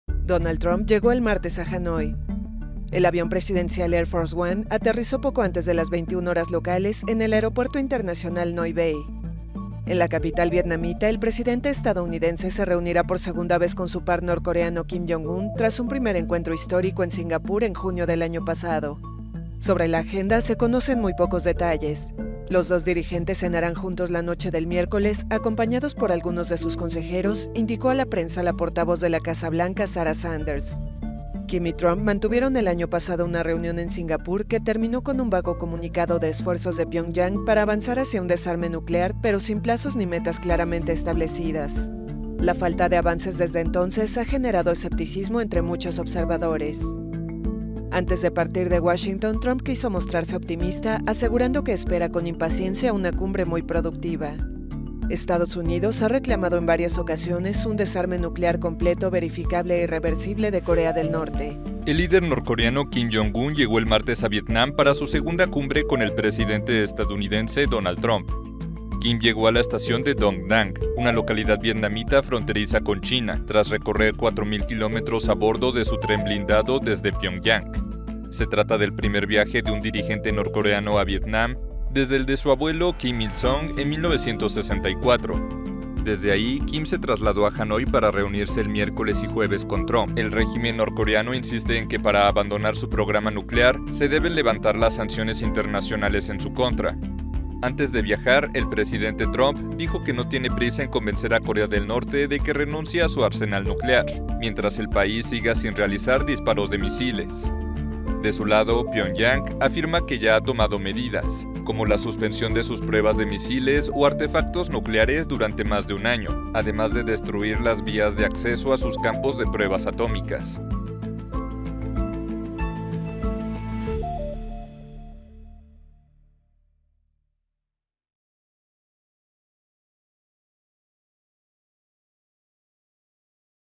Donald Trump y Kim Jong Un ya están en Vietnam listos para la cumbre I EL TIEMPO
Se esperan acuerdos importantes en el proceso de desnuclearización de Corea del Norte.